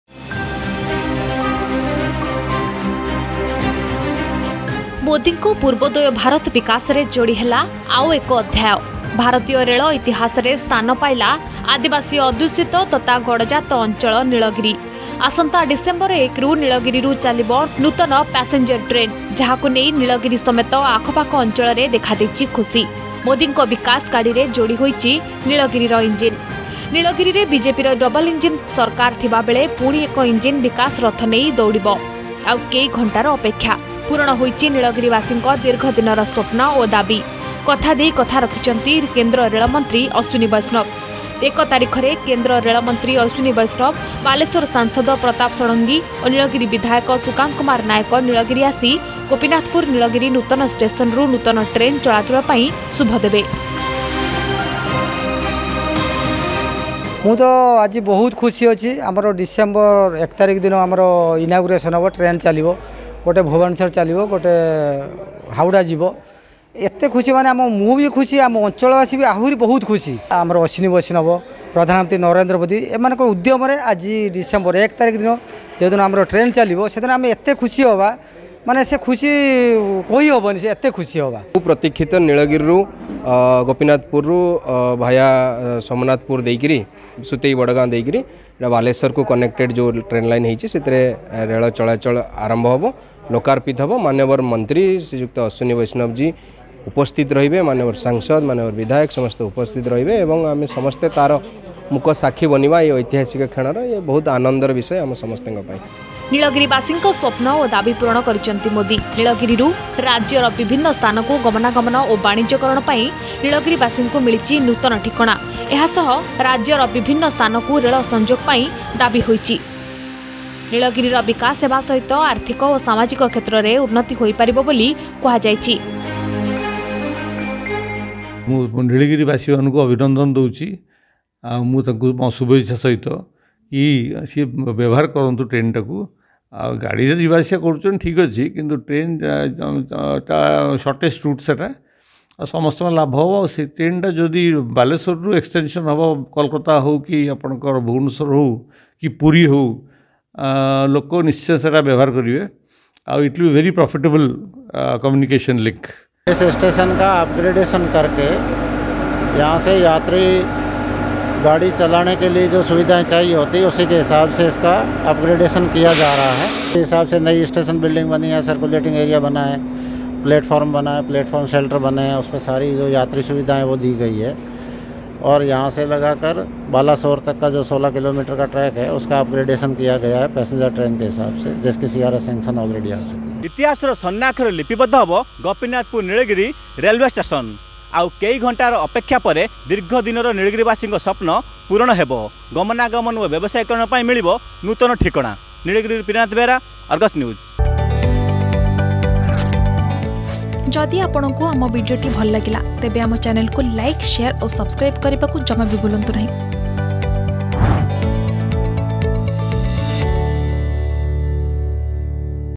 Passenger Train Service To Nilgiri To Begin From December 1
This will bring economical and social change to the region.